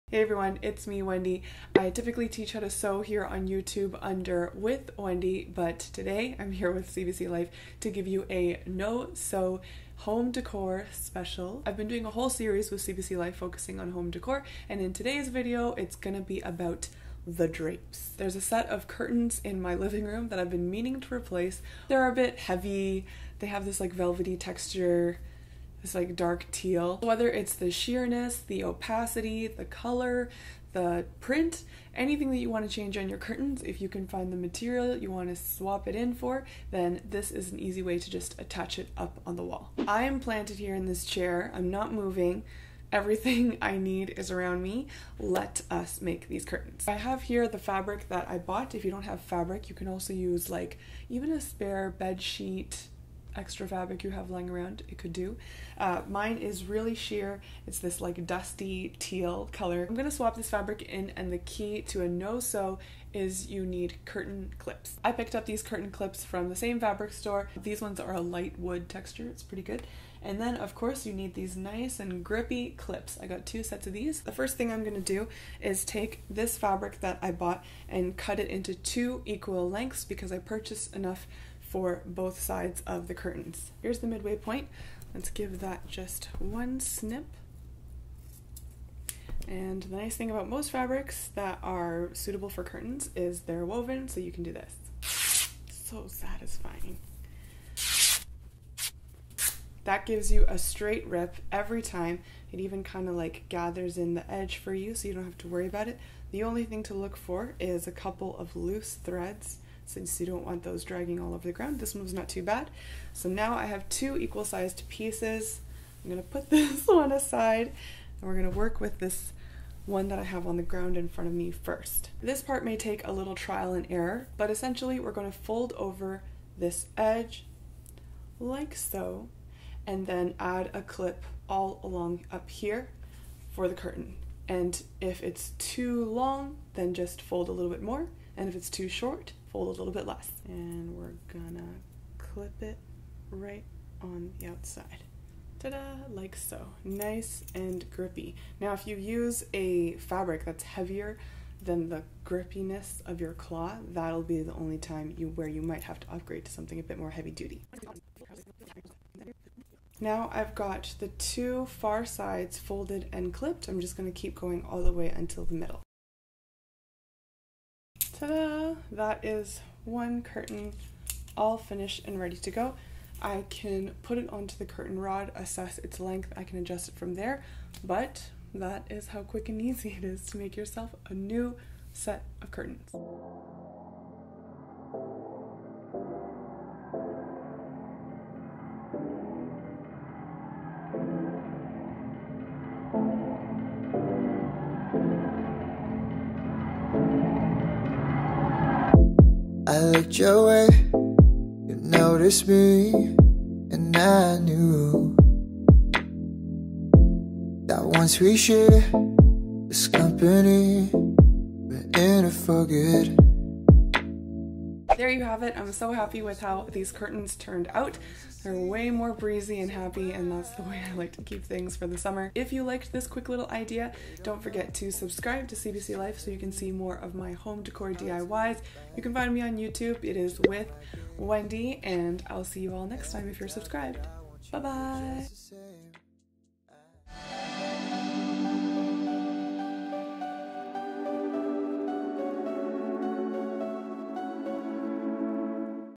How to Create DIY No Sew Curtains | withwendy
This simple, stress-free DIY no sew curtains project created by withwendy will yield some high-impact results.